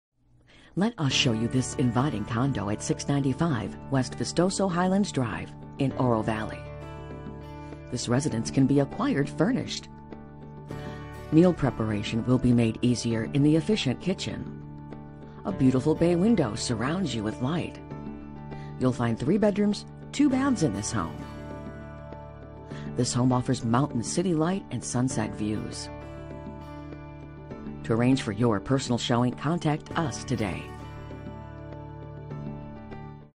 695 W Vistoso Highlands Dr #206 Oro Valley, AZ 85755
It really doesn't get any better than this. Sought after 2nd floor 1,730sf 3BR/2BA Vistoso Casita overlooking Oro Valley's Vistoso Trails Nature Preserve w/Mountain, City Lights & Sunset Views. Rarely available, this casita can be sold fully furnished ''Turn-Key'' w/upscale furnishings, electronics, household & decor items included. Master suite has an alcove area w/access to the covered balcony. This casita has never been used as a rental and is in pristine condition from minimal use. Maintenance-free lifestyle w/HOA that includes trash removal, water, streets, exterior paint, roof & pest control. This fantastic Oro Valley location has immediate access to some of the best outdoor activities available, adjacent Oro Valley's Vistoso Trails Nature Preserve, featuring a 6.2-mile, eight-foot-wide concrete path that winds through nature's beauty & Oro Valley's finest amenities. Next to Stone Canyon, you have options for golf & social memberships, opening the door to world of exclusive privileges. Golf Membership grants access to the championship golf course and health/fitness facility & privileges at the clubhouse, dining facilities, locker rooms & participation in all club functions & activities. Sports membership offers the same incredible benefits, excluding golf. Newer Heat Pump & water heater. Attached garage. Original owner. Enjoy this casita yourself year-round, part-time or short-term rental opportunities are available.